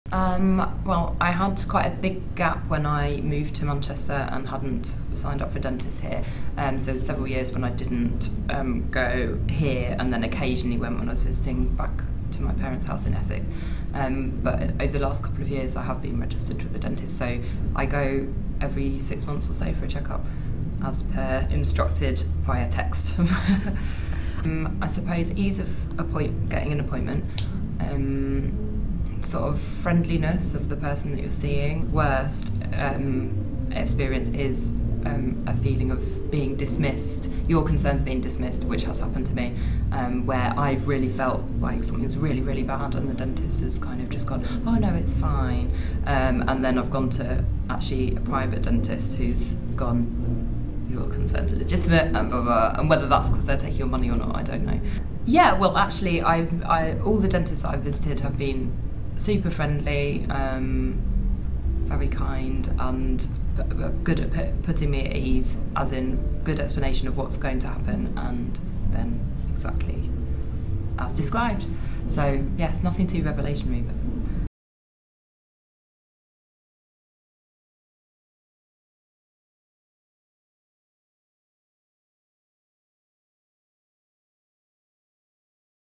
Kathryn, Dentist Story
This film has been made as part of a Manchester University and Salford Royal study into people's experiences of quality dentistry.